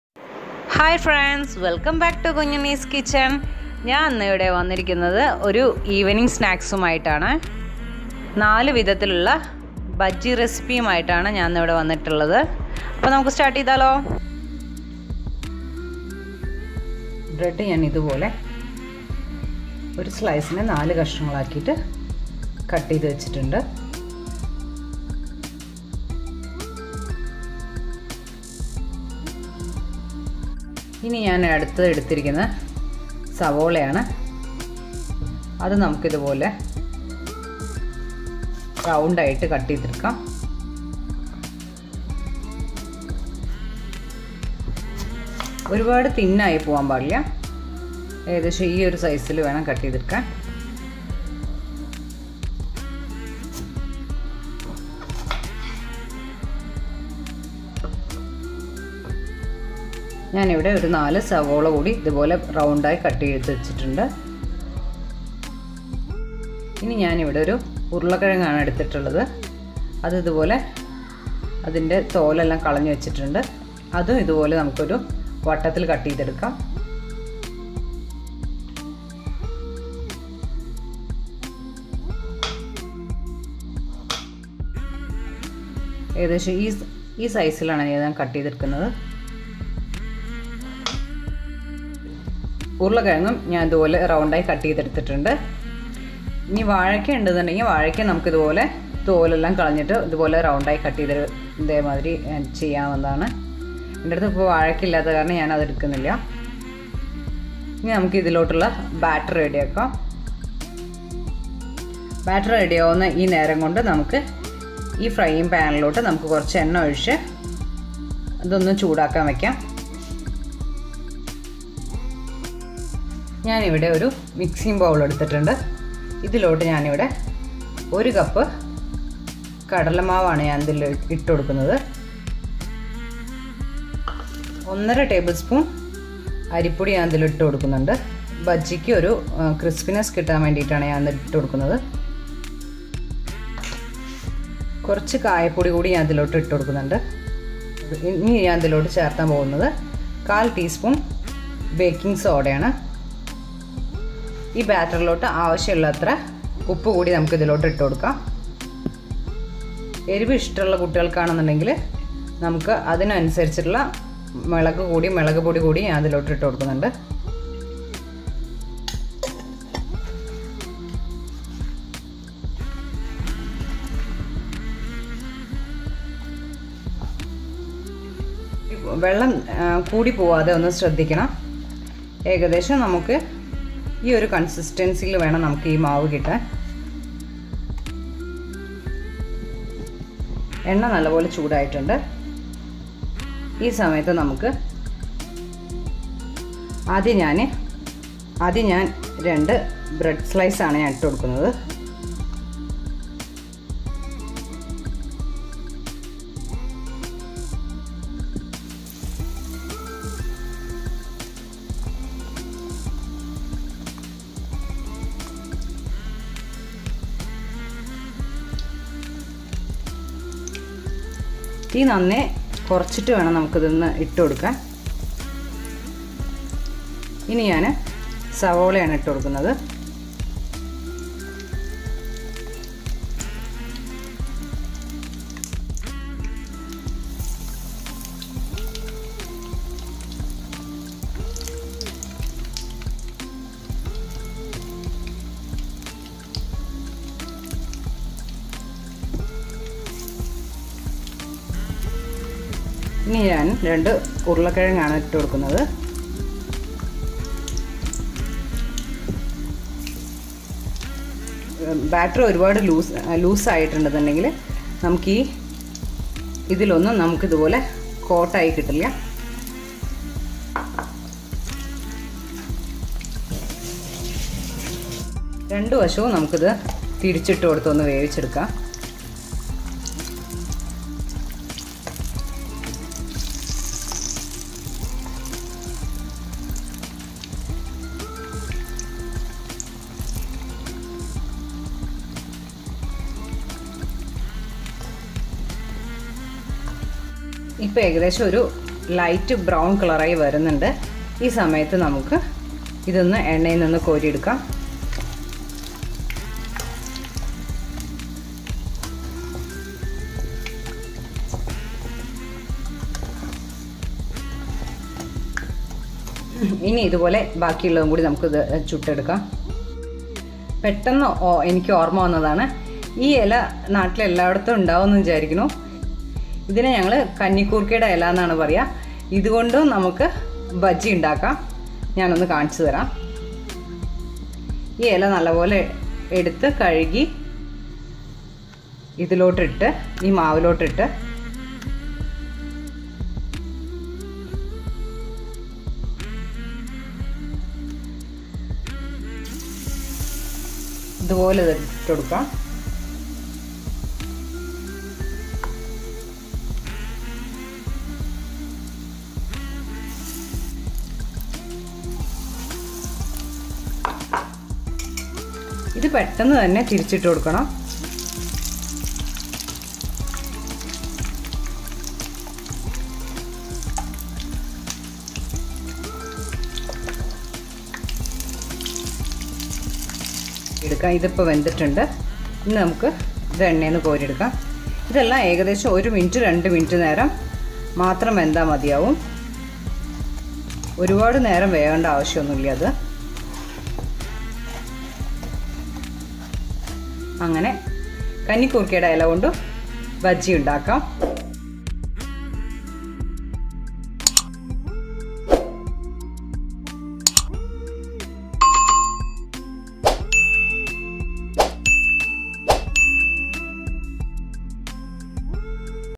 4 types of bajji recipes // easy evening snacks
Bread
Onion
Potato
Oil
Gram flour
Chilli powder
Baking soda
Salt